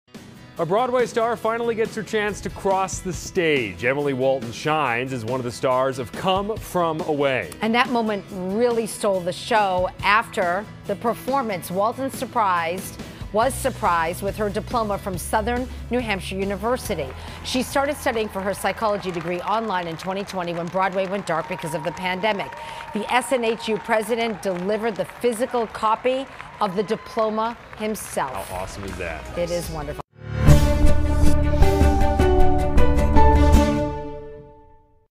Broadway star surprised on stage with her college diploma from Southern New Hampshire University
The president of the university surprised the actor on stage also brought a cap and gown for her to wear.